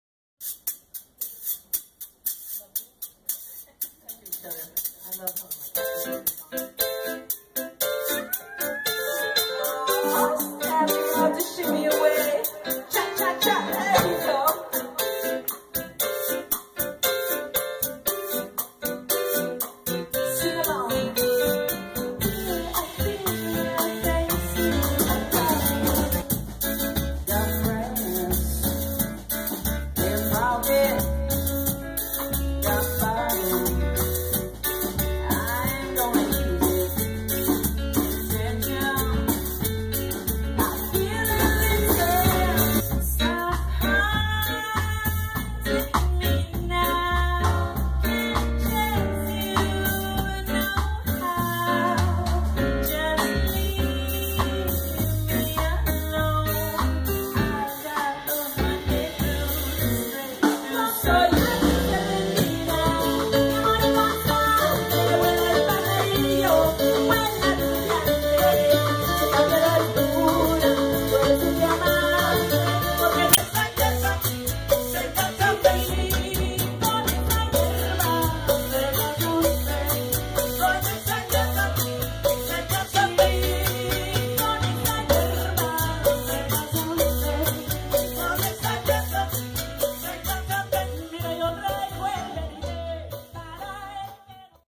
Cava at Zona Rosa Coffee
Walter Miranda, Cava, and Adam Topol of the band Cava, joined by Gabriel Gonzalez (maracas), play their “ceviche gumbo” mix of spicy latin-afro-cuban-funk-fusion music at Zona Rosa Coffee adjacent alley on Sunday, September 13, 2015.
Story/Video Monique A. LeBleu